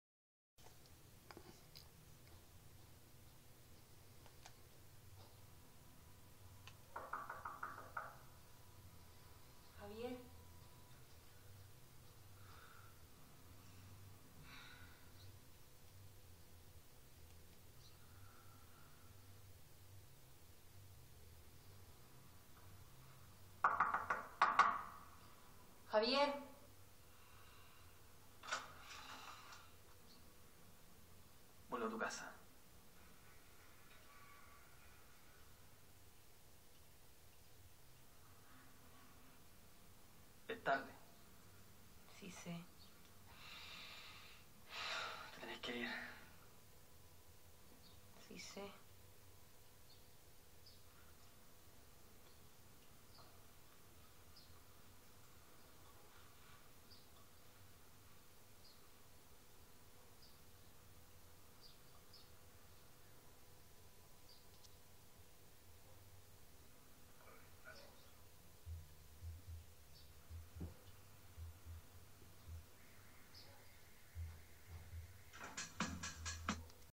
Omar Méndez - Actuacion besos de series chilenas.
Uno de mis primeros trabajos como actor en televisión, en conjunto con alumnos de Comunicación audiovisual de la Uniacc. Copia fiel correspondiente a una escena de una serie Chilena.